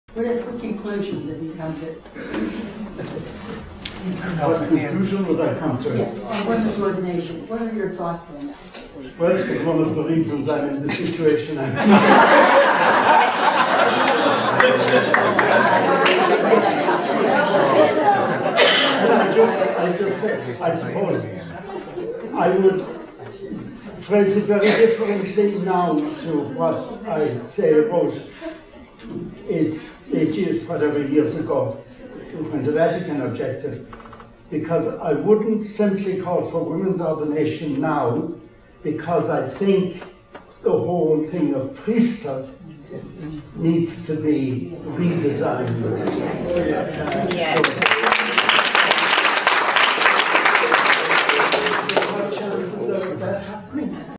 Tony Flannery discusses the ordination of women
Tony Flannery discusses the ordination of women in the context of the meaning of the priesthood.